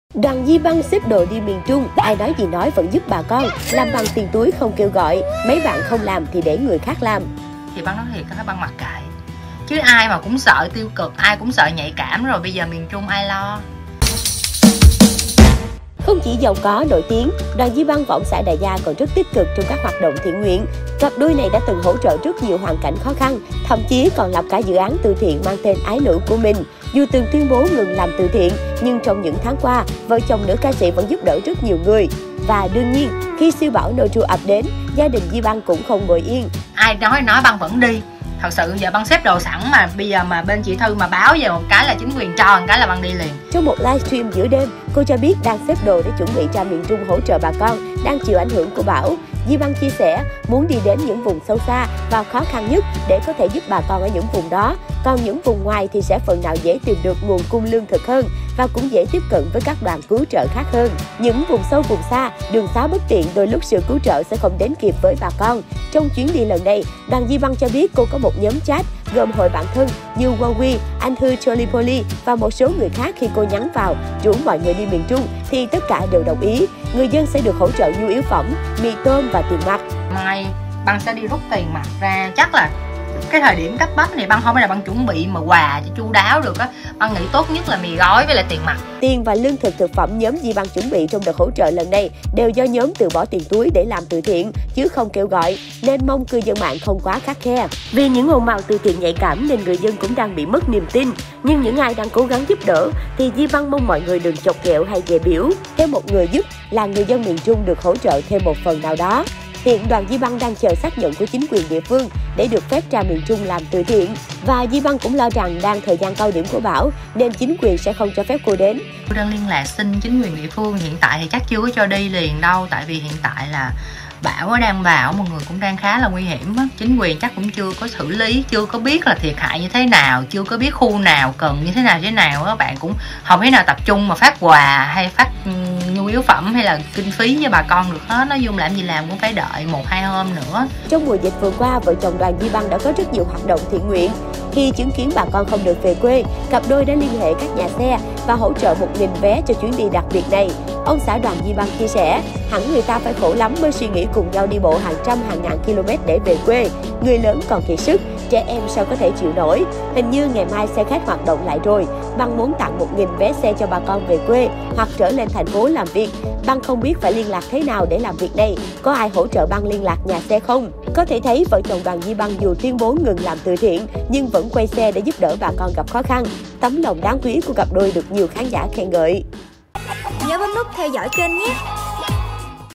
Đoàn Di Băng bỏ tiền túi ra miền Trung làm từ thiện: "Ai cũng sợ nhạy cảm thì miền Trung ai lo"
Hãy theo dõi Yan trên:
✔️Youtube Yan News :